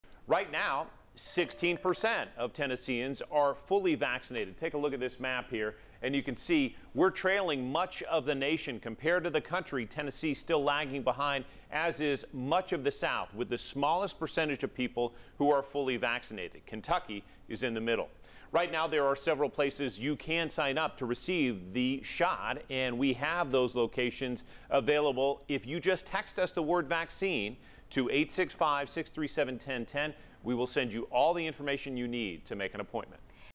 TDH: 16% of Tennessee fully vaccinated
Compared to the rest of the country, Tennessee, and much of the south, have the smallest percent of people who are fully vaccinated.